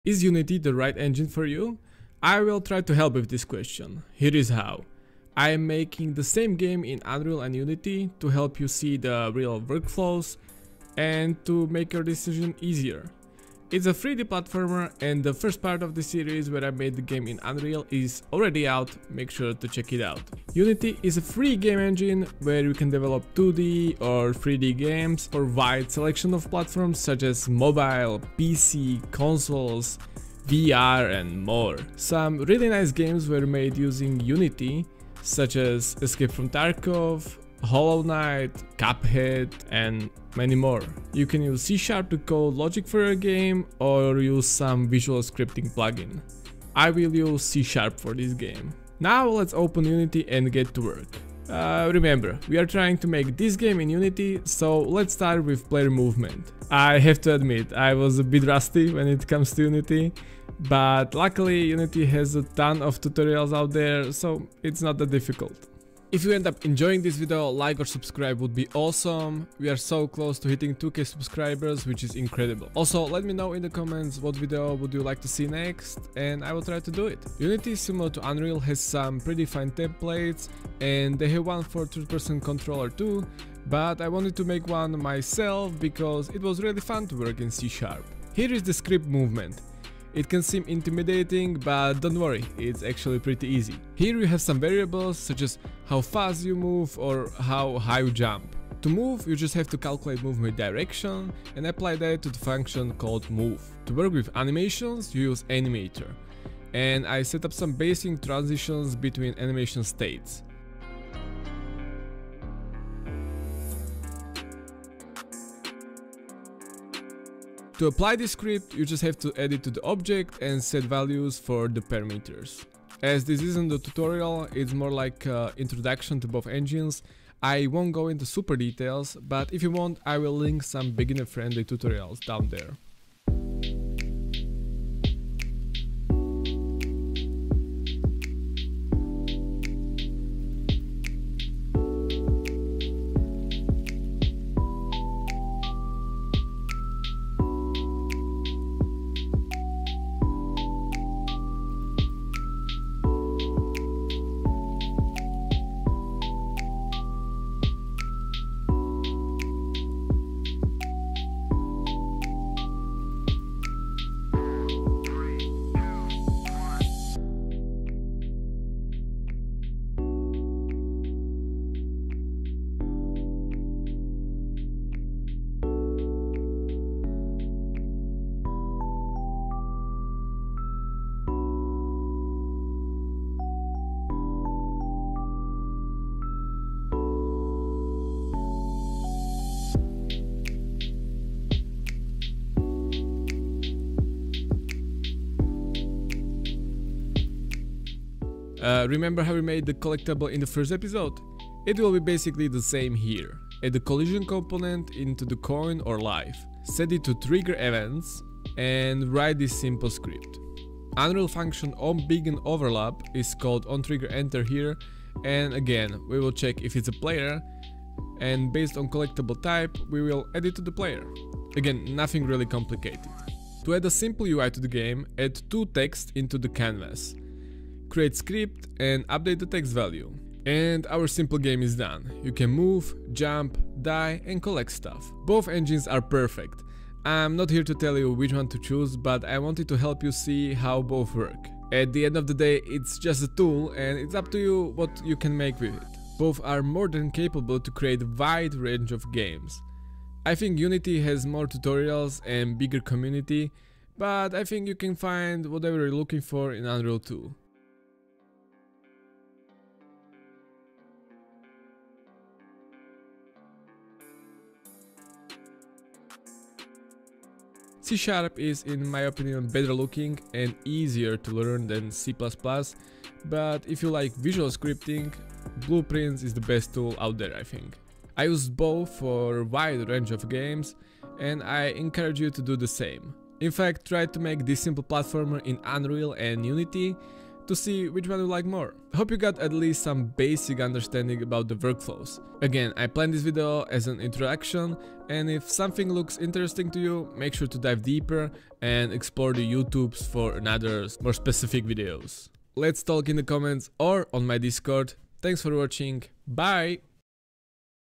Unity or Unreal? Same game - 2 engines #2 Unity3D
Unity or Unreal Engine?  - How to start with game dev? - Which game engine is the right one for you?  Hopefully this video will help you decide! I will be making the same game using unity 3D and unreal engine 4. You will see real workflows and learn a bit from both engines to help choose the right one for you. In this part I made the game in Unity3D.

Channels you might enjoy!

Unity or Unreal? Same GAME 2 ENGINES